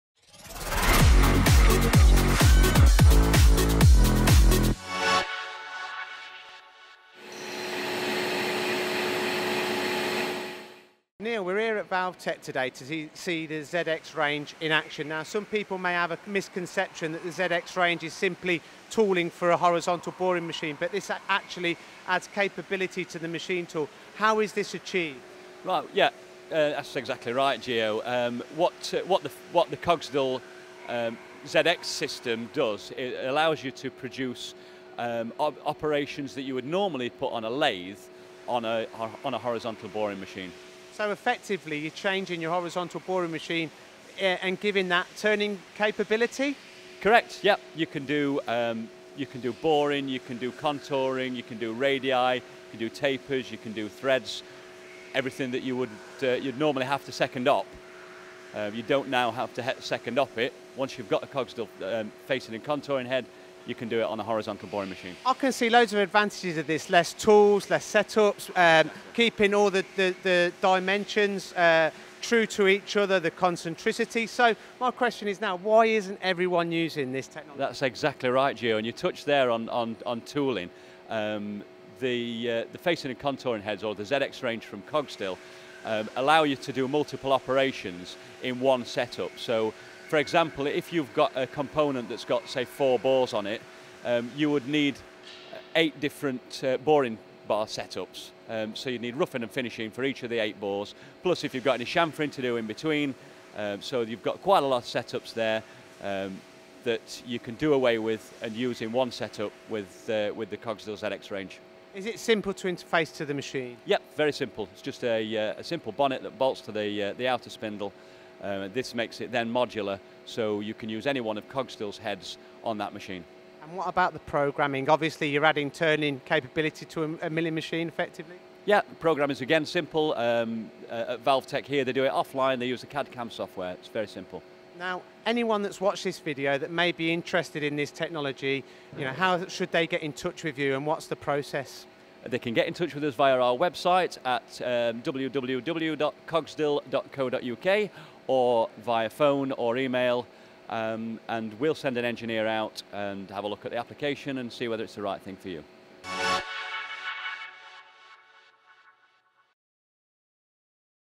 Cogsdill - Adding turning capability with a ZX300 Contouring Head
An interview with Neil Yewman from Cogsdill on-site at Valvetek Engineering in Huddersfield where capacity has been increased with the introduction of a Cogsdill ZX300 Facing and Contouring Head.